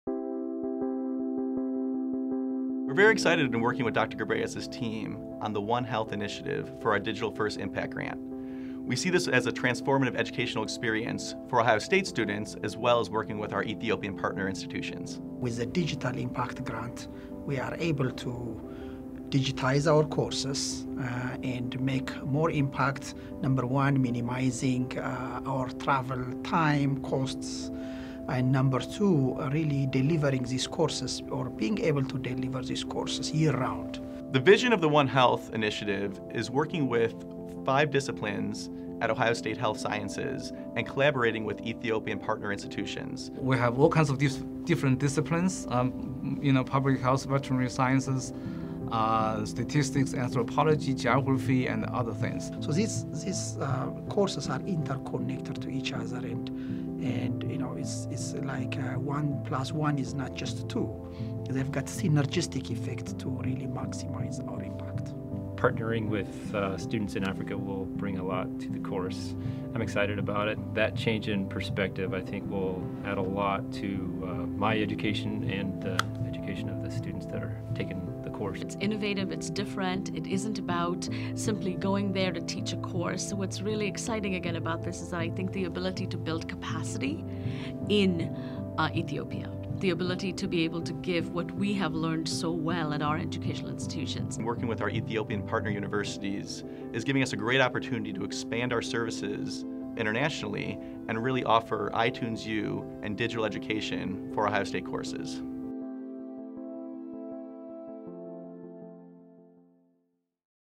Digital First Impact Grant: Global OneHealth Initiative
Dr. Wondwossen Gebreyes and a team of instructors discuss their Global OneHealth Project that kicked off on Dec. 4, 2014. This project, funded by an ODEE Digital First Impact Grant will provide funding for 40 iPad minis to conduct teaching, research, and gather data at Ohio State and with multiple university partners in Africa. ODEE awards one Digital First Impact Grant per year to enhance the student learning experience through mobile educational resources and technology.